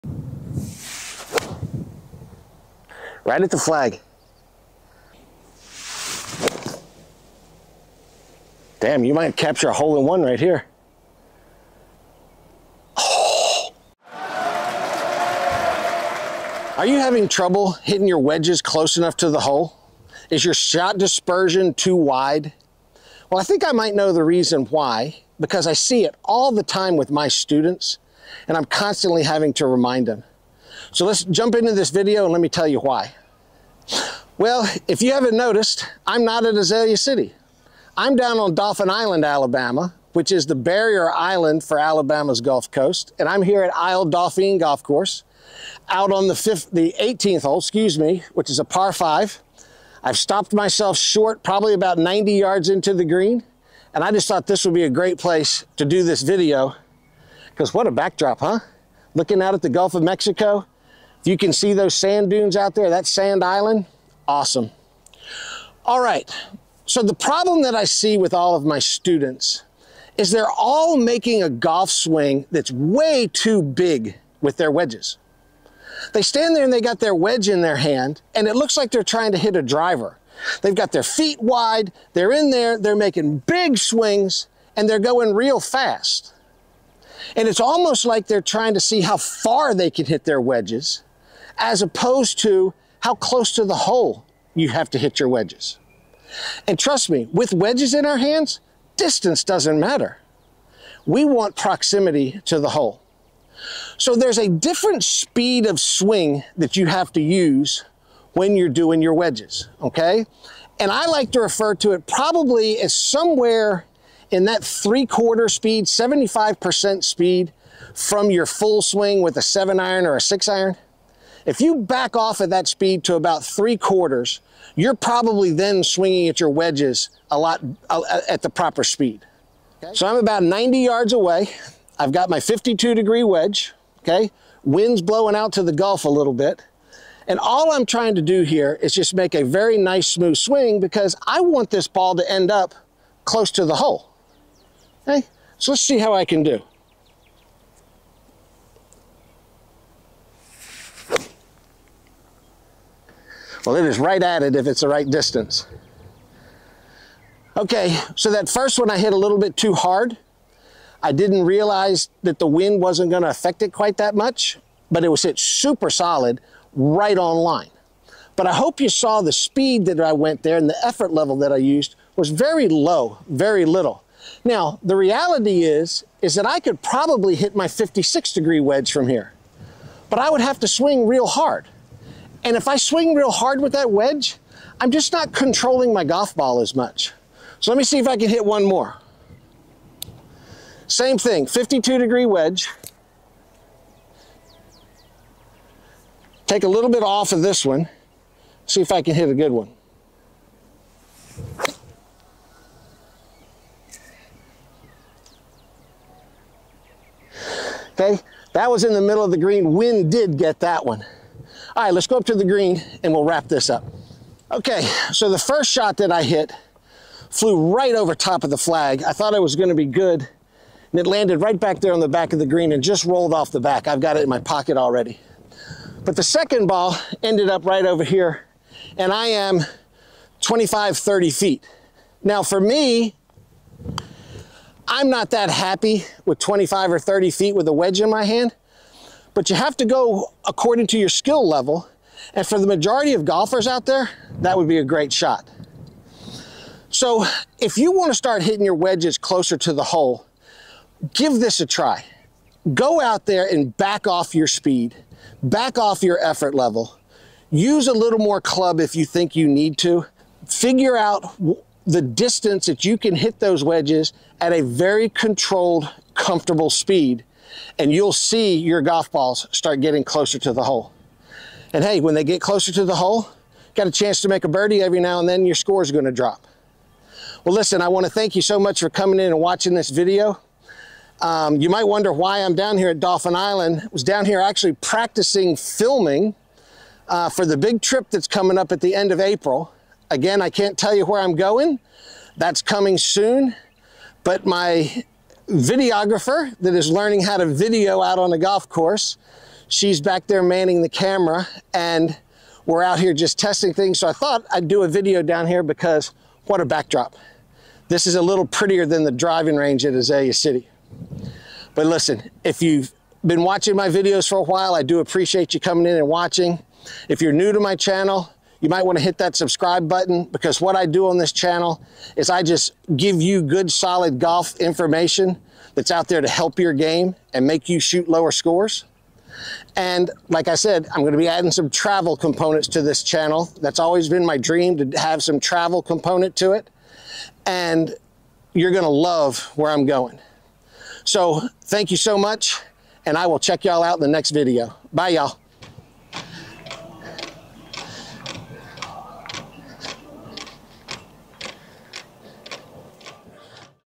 How To Hit Your Wedges Closer | More Accurate | Lower Golf Score
Learn how to hit your wedges closer to the hole and shoot lower golf scores. Creating a more consistent golf swing with your wedges will make you more accurate. Improve your golf swing and improve your golf score.